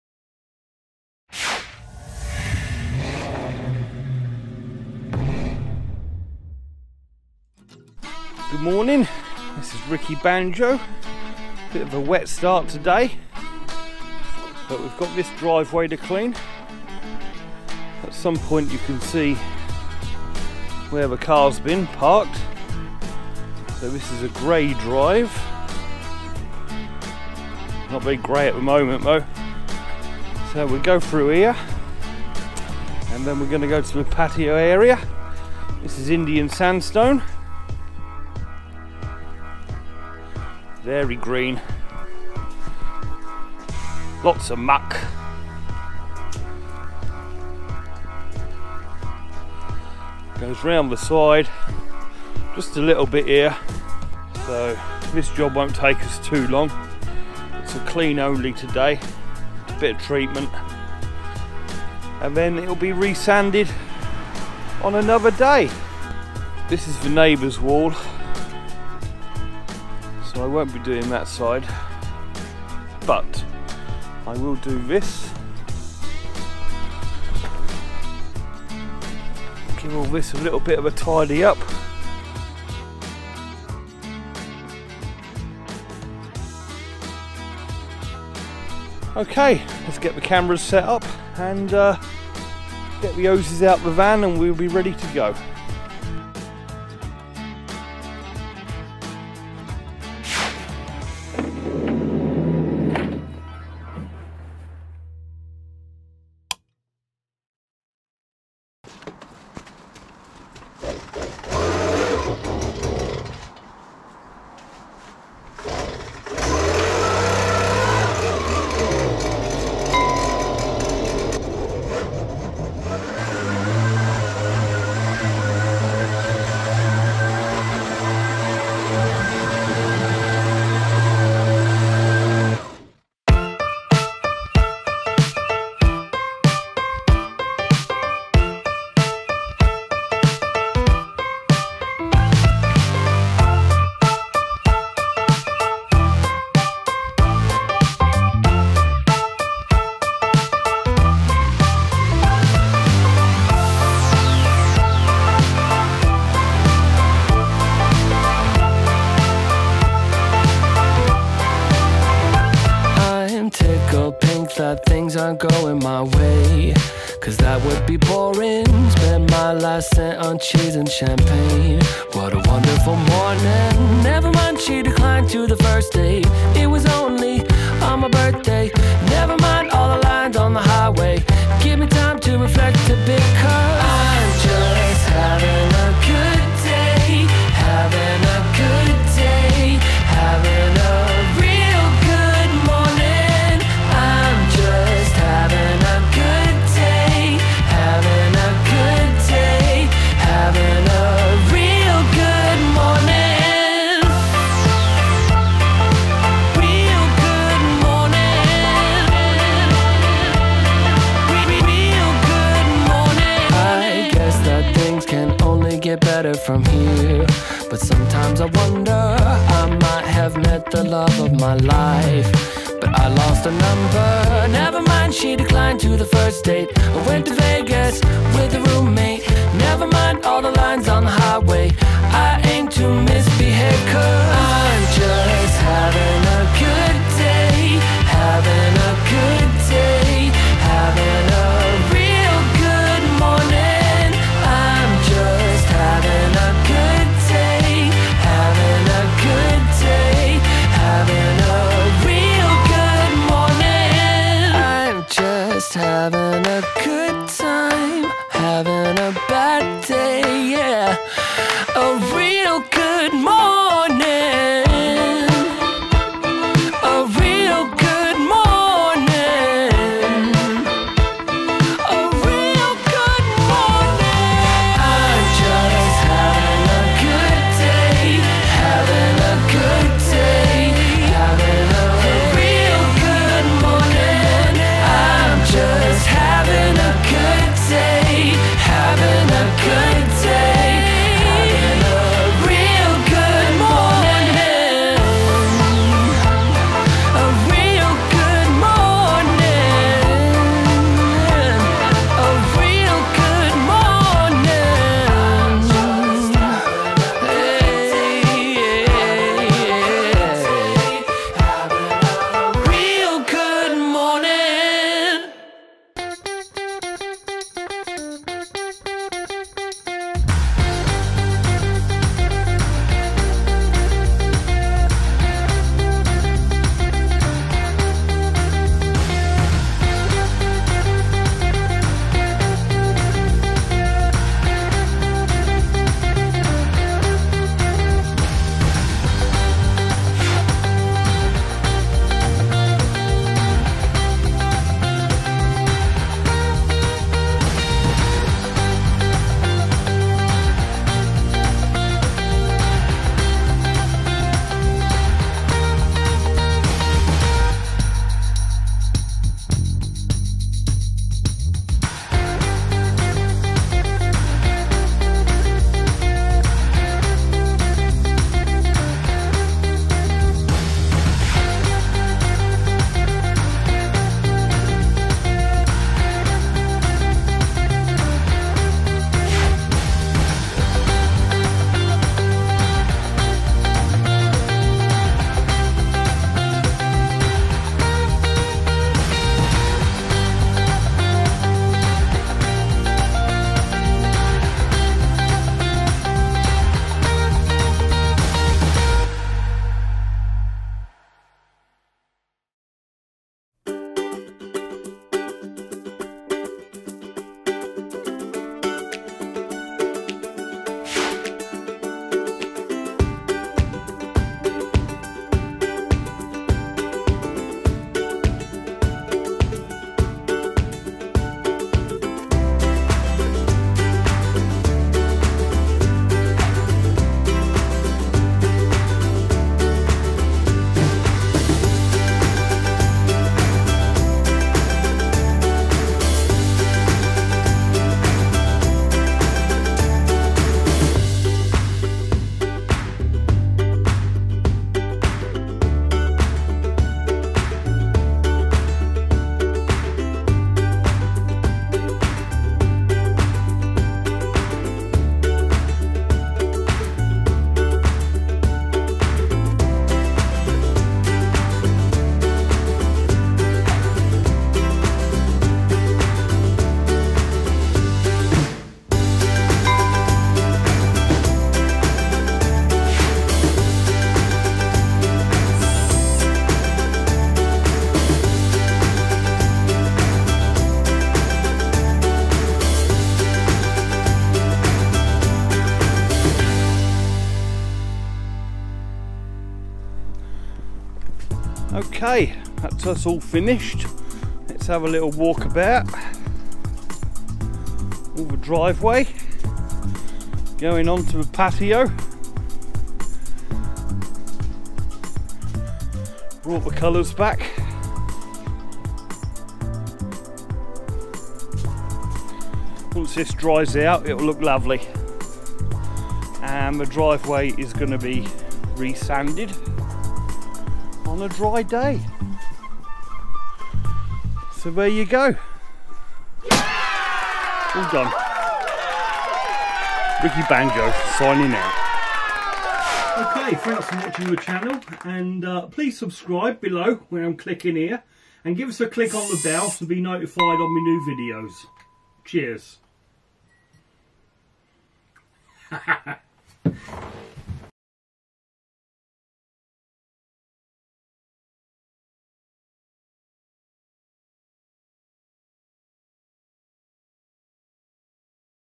Driveway and patio cleaning
Just a small driveway and patio pressure clean today, The customer is a local builder and is going to resand it when it is dry. Another happy cutomer using bournemouth jet washing for there driveway and patio cleaning service.

✅ Websites